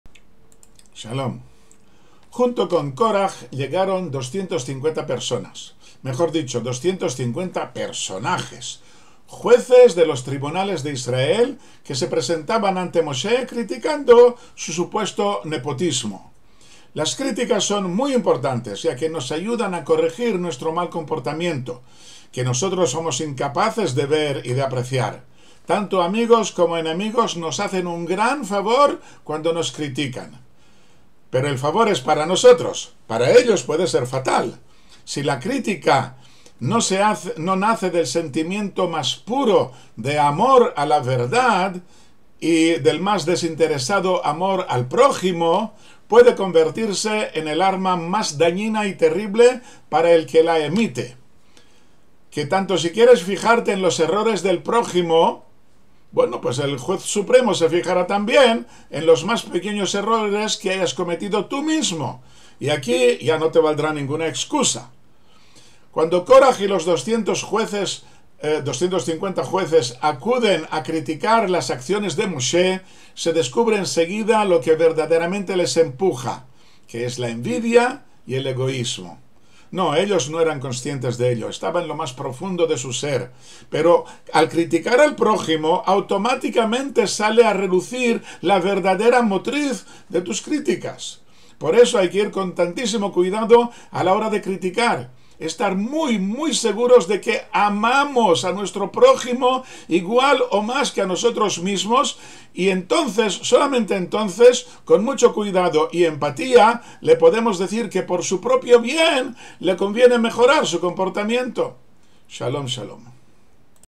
El Amor a la Verdad
Comentario a la parashá de Córaj (Números 16:1 - 18:32)